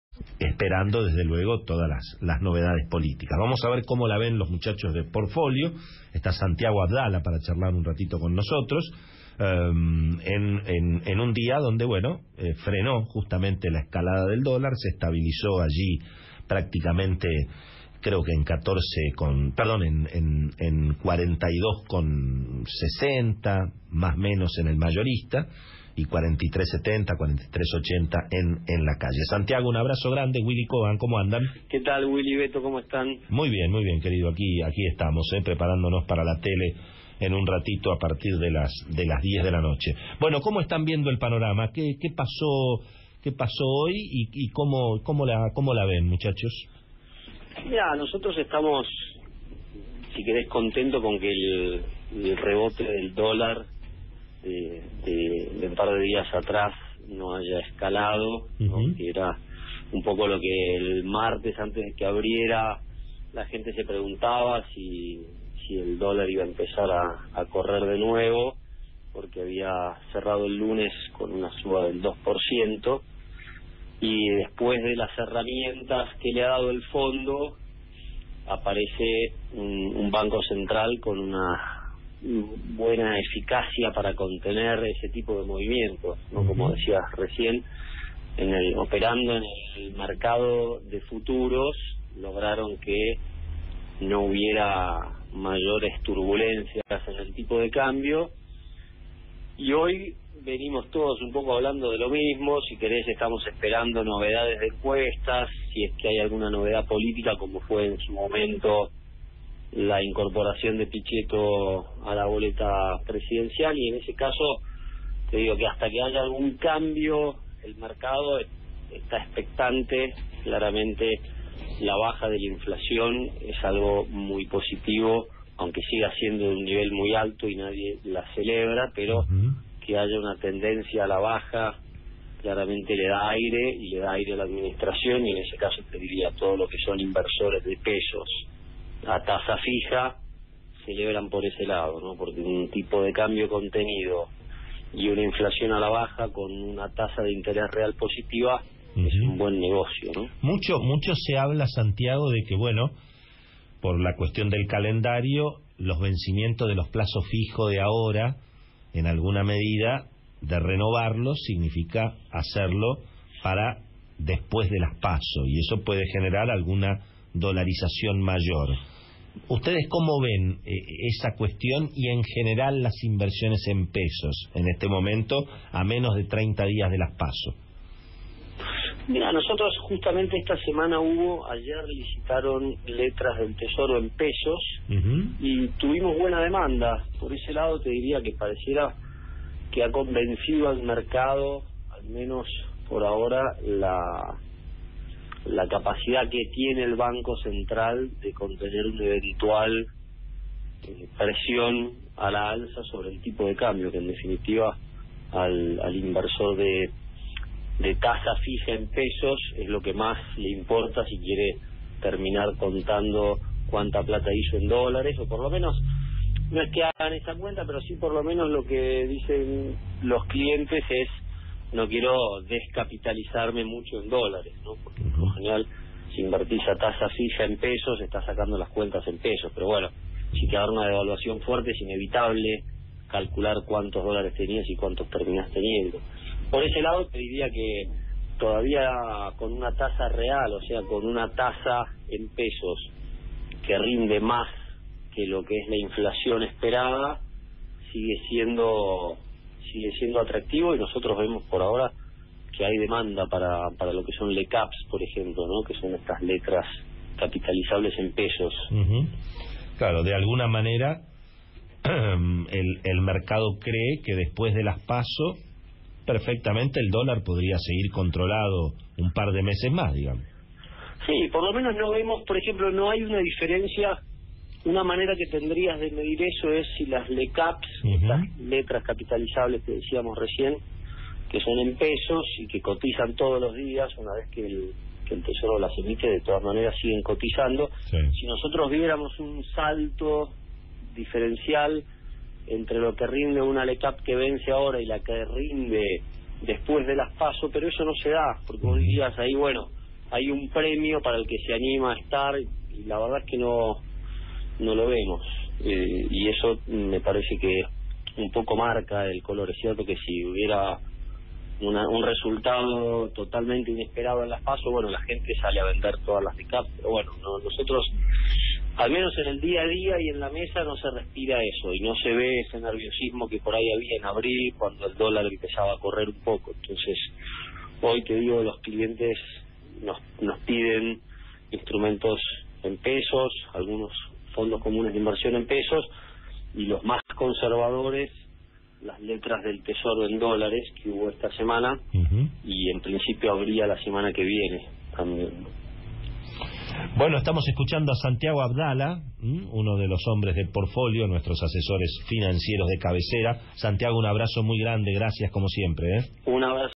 Análisis de Coyuntura y Recomendaciones
Nuestro Director, Santiago Abdala, conversó con Willy Kohan y destacó "Estamos contentos con que el rebote del dolar un par de días atrás no haya escalado. Después de las herramientas del Fondo, aparece el Banco Central con una buena eficacia para contener este tipo de movimientos."